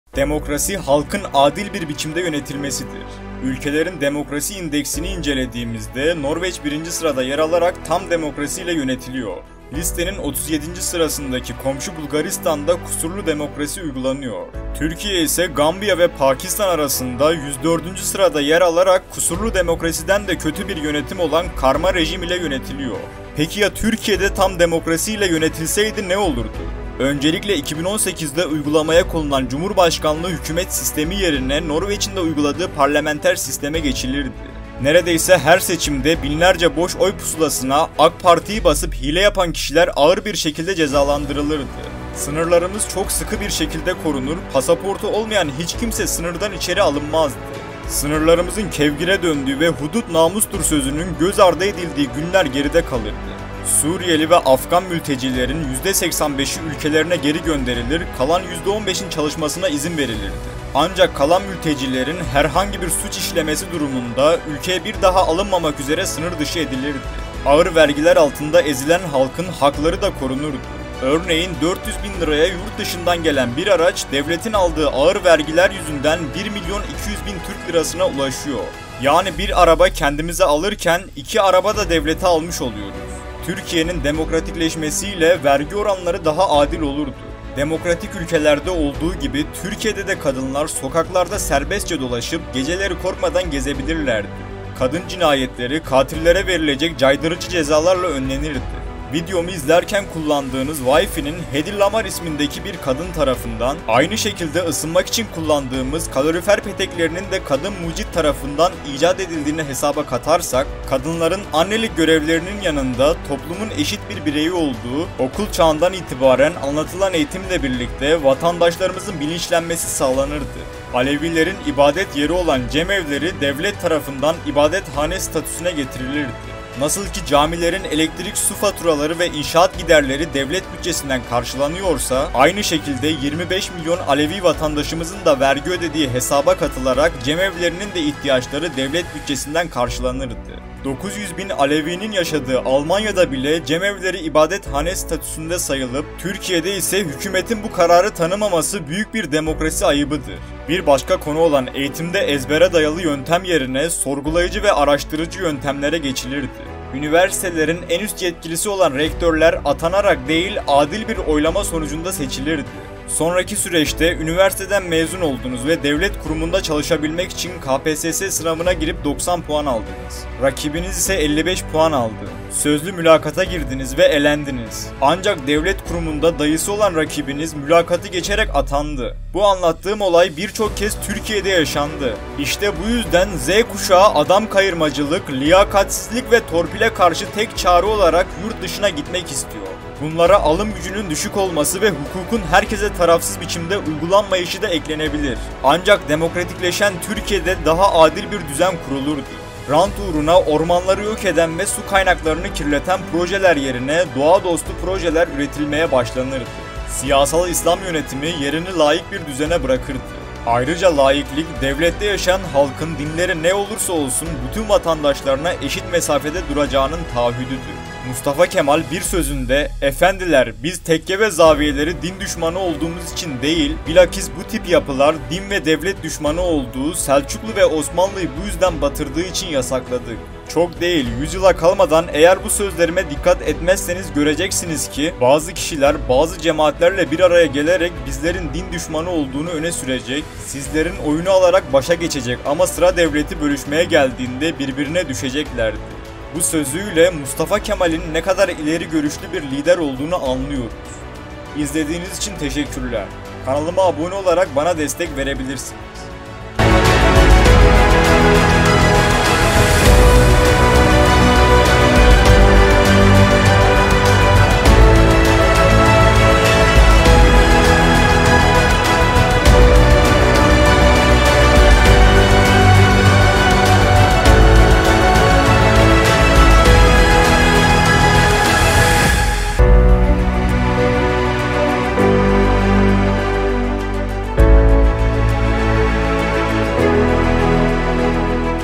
Türkiye Demokrasi ile Yönetilseydi?
Bu videomuzda Türkiye'nin tam demokrasi ile yönetilmesiyle ülkede ne gibi değişiklikler gözleneceği ele alınmıştır. İyi seyirler.